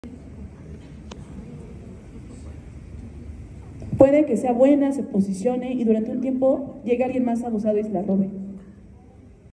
I-konic
Nuestra querida amiga Claudia V en magistral conferencia mencionando a KRJF y la Agencia de Marketing y Publicidad I-Konic